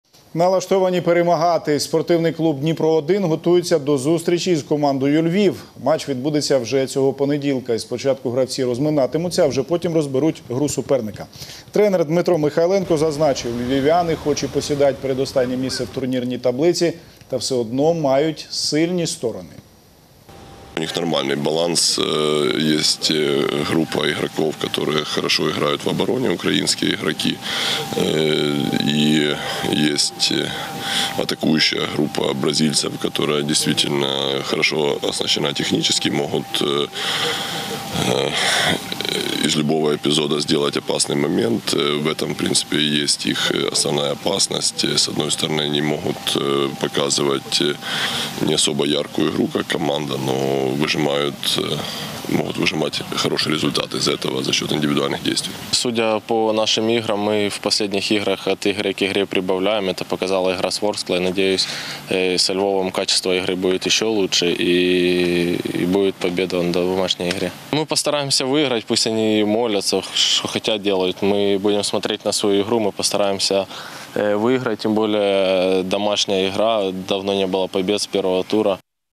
Готовы давать бой: СК “Днепр-1” настраивается на встречу со “Львовом”
Спортивный клуб “Днепр-1” начал подготовку к домашней игре со “Львовом”, которая состоится в понедельник, 26 августа.